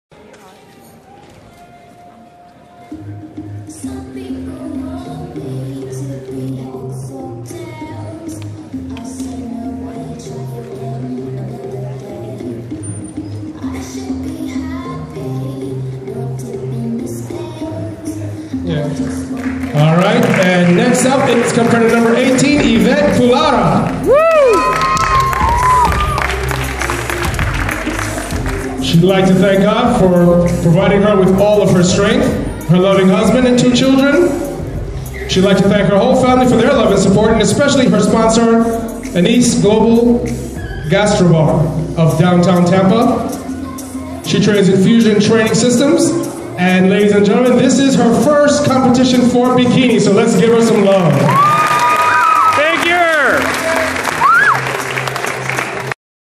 First Figure Competition Natural Suncoast Classic
May 2015, My very first figure show ever!